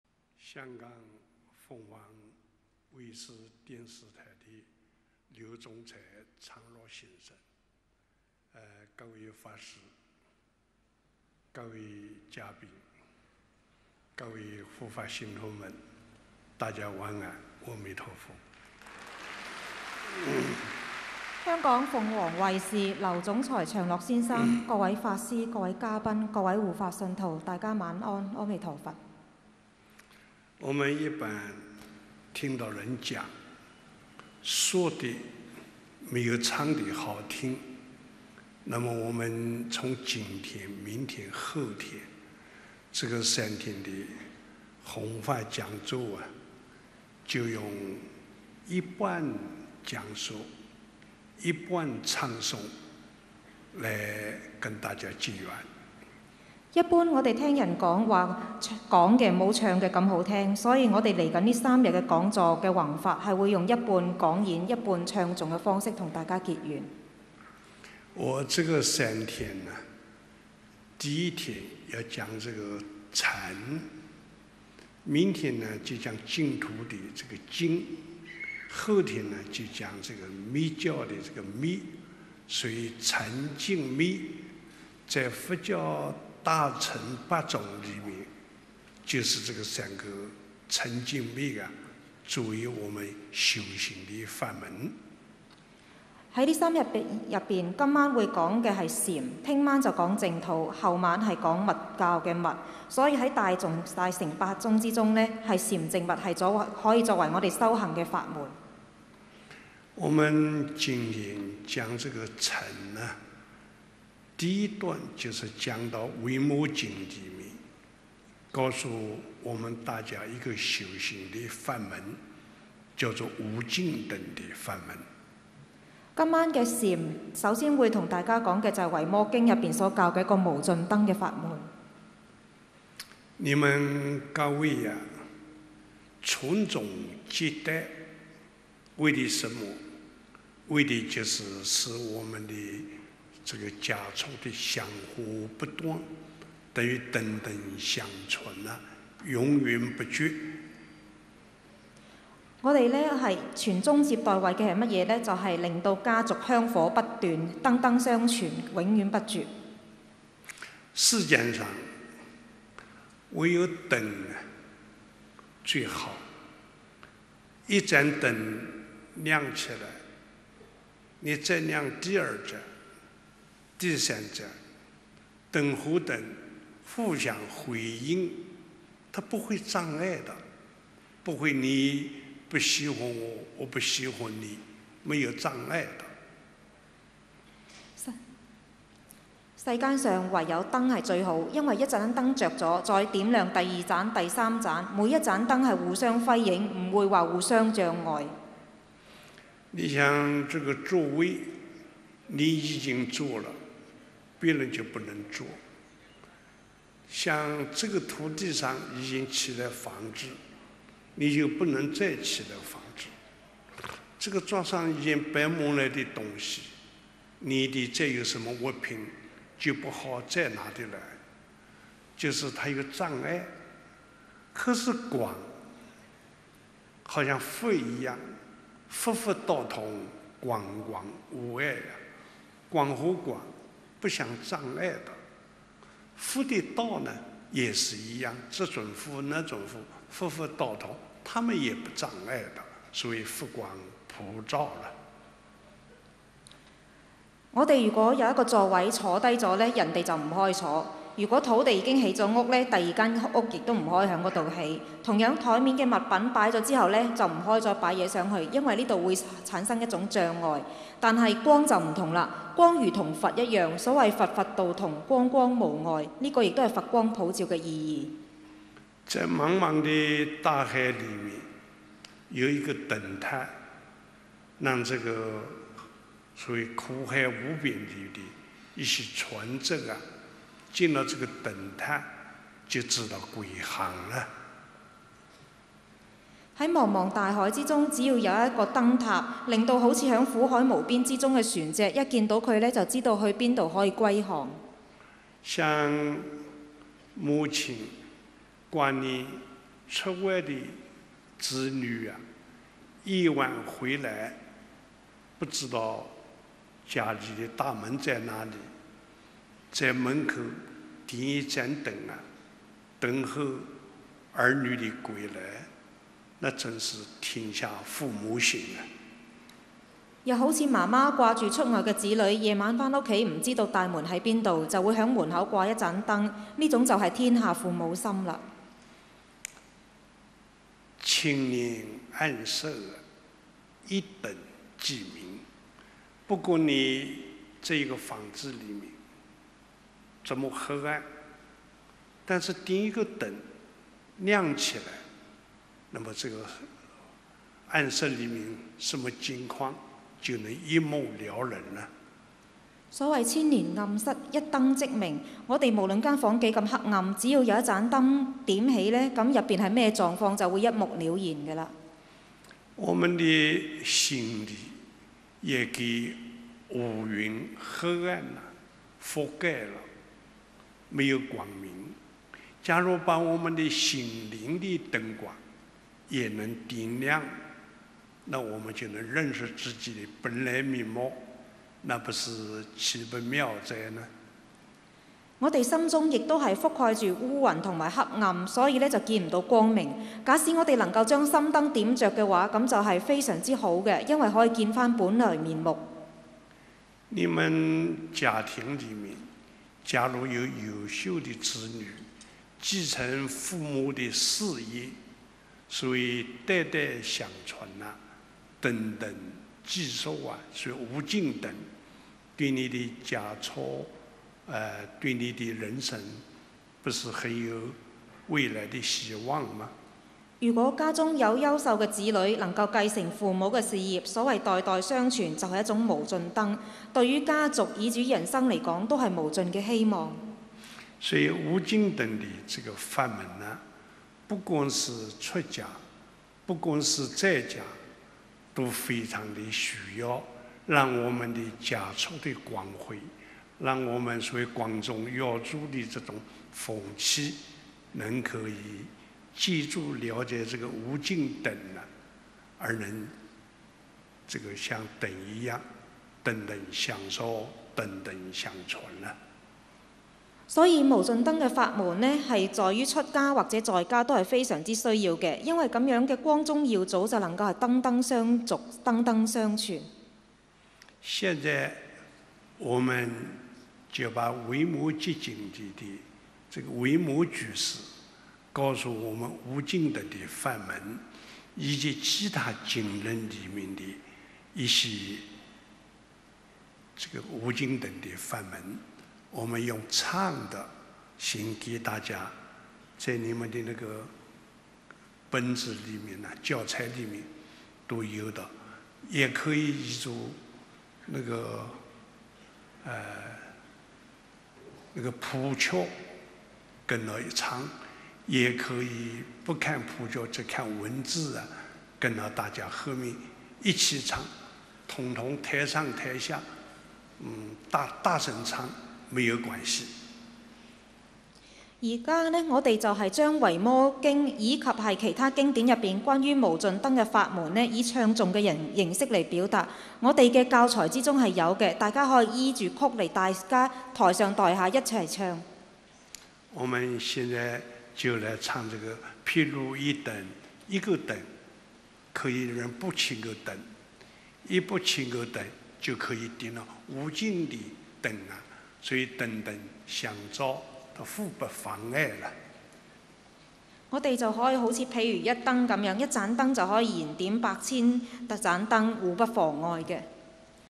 星雲大師佛學講座_禪淨密唱頌講座(一)_(1)
2003年，星雲大師於香港紅館舉辦佛學講座，為期兩天，第一天講授主題為「禪淨密唱頌講座(一)」。